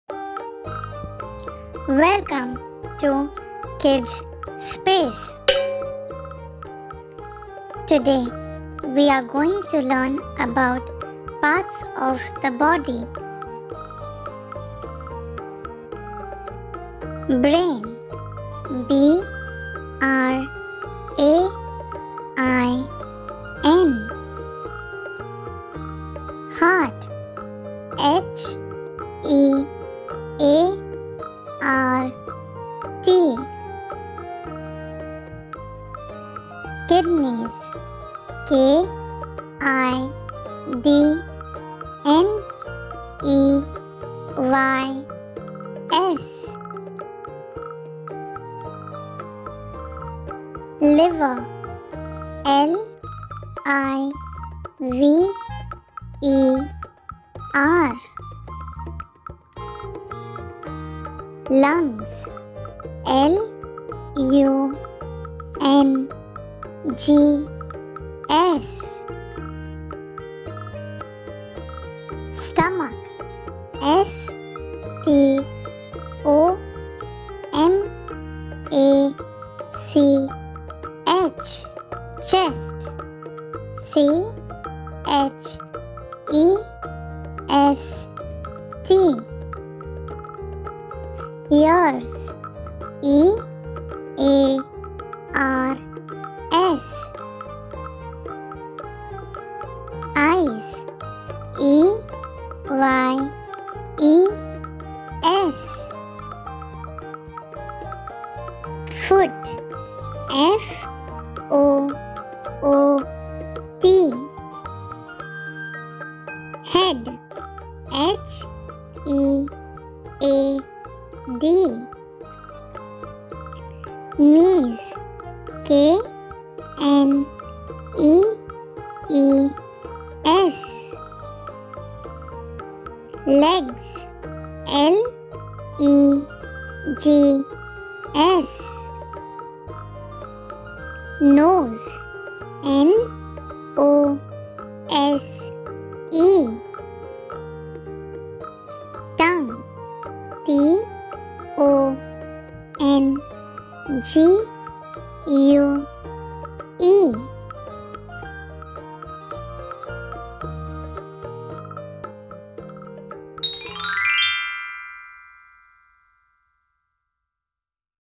PART'S OF THE BODY NAME ! Learn about Part's of the Body Internal and externa !
In today's Video we are going to learn about Part's of the Body Internals and external.